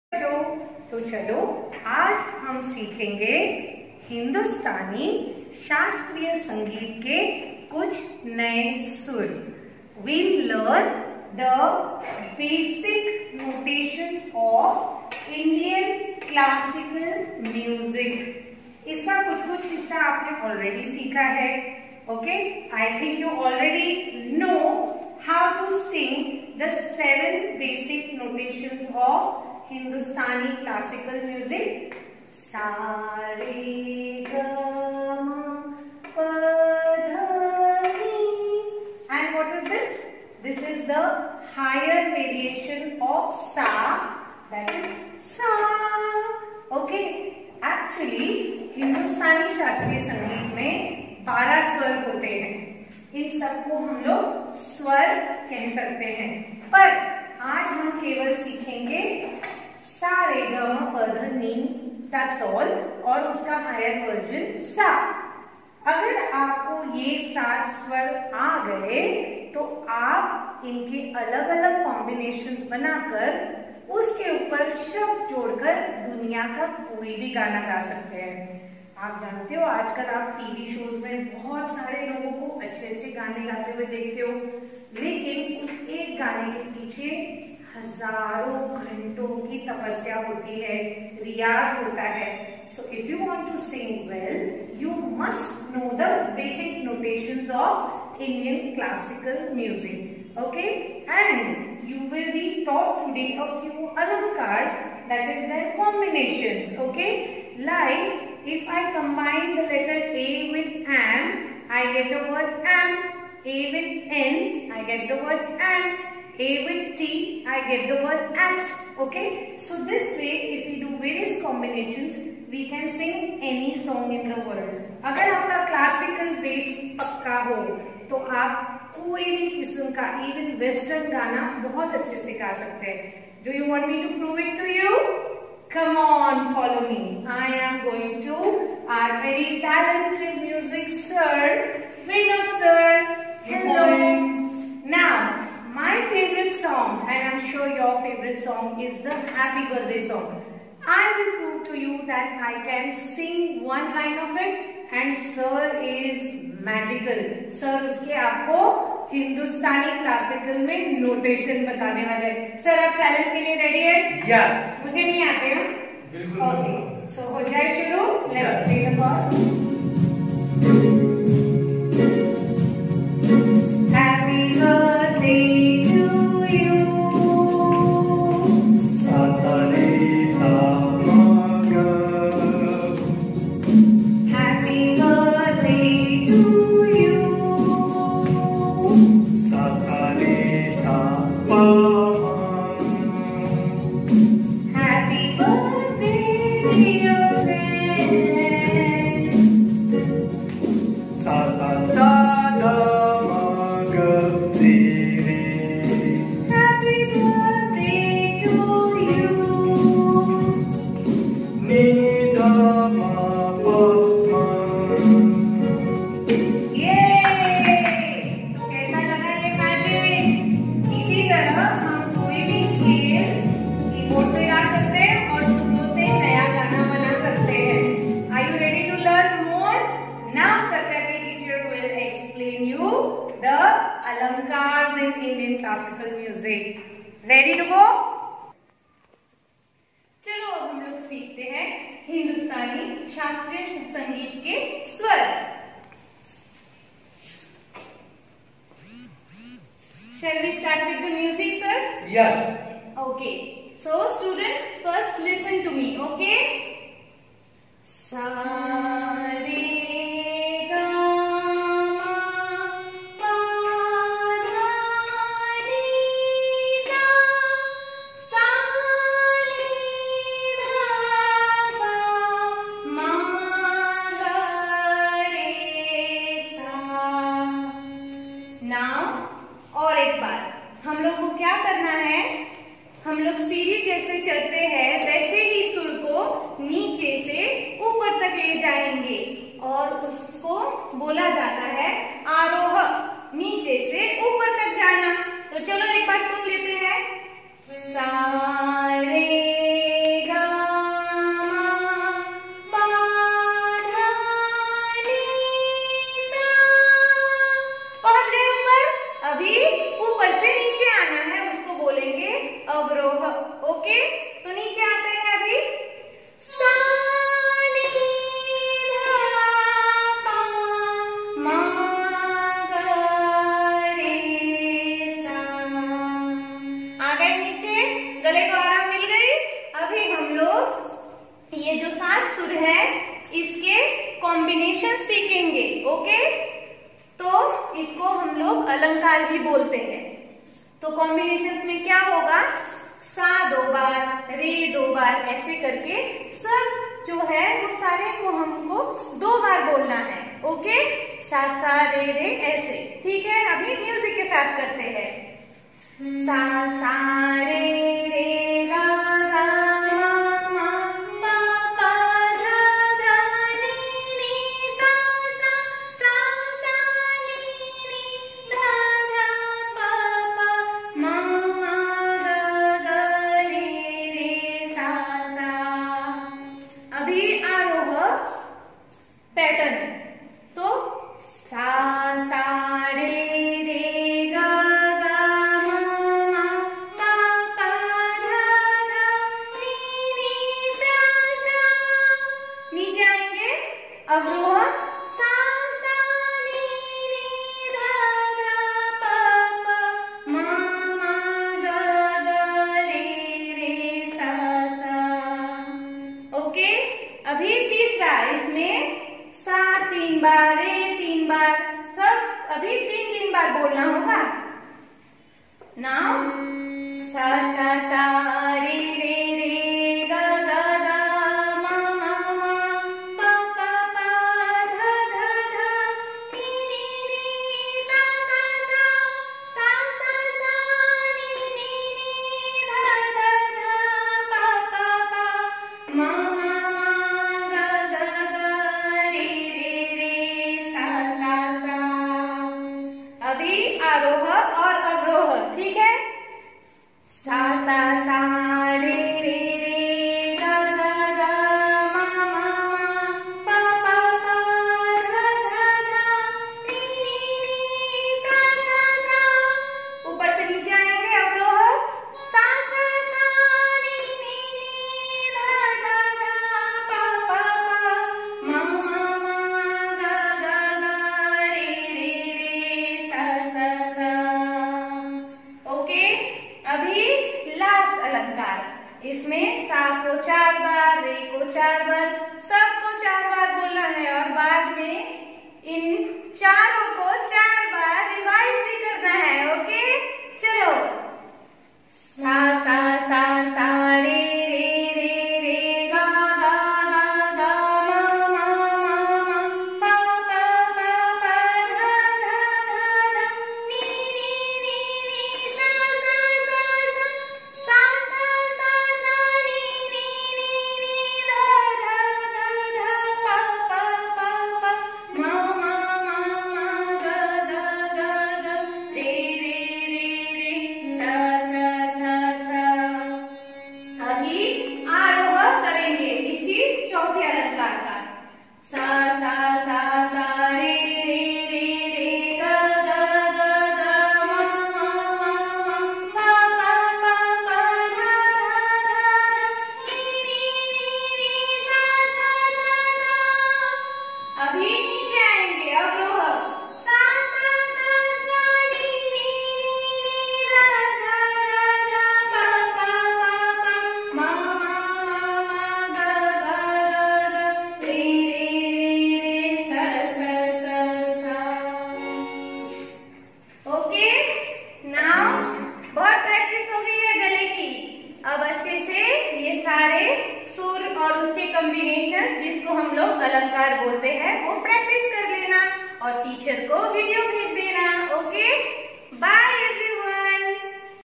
Music # 1 Introduction to Indian Classical Music
Basic 7 Notations ie Shuddh Swars and three Alankaars of Hindustani Shastriya Sangeet for beginners.